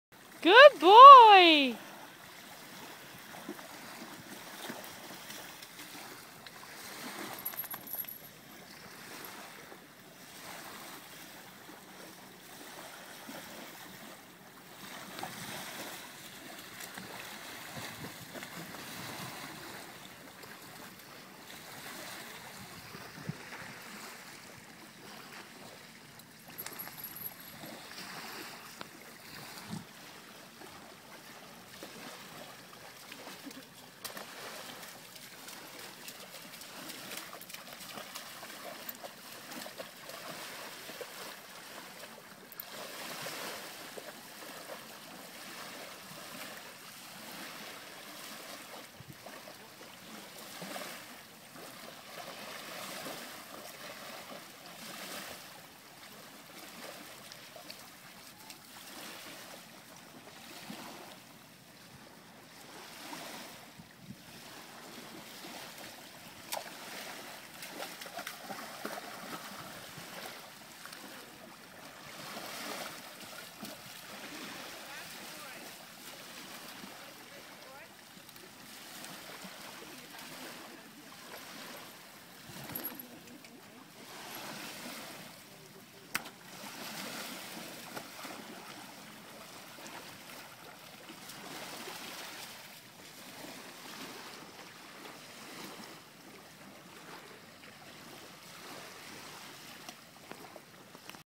English Bay stick fetch
Sam and Michael play stick in the water by English bay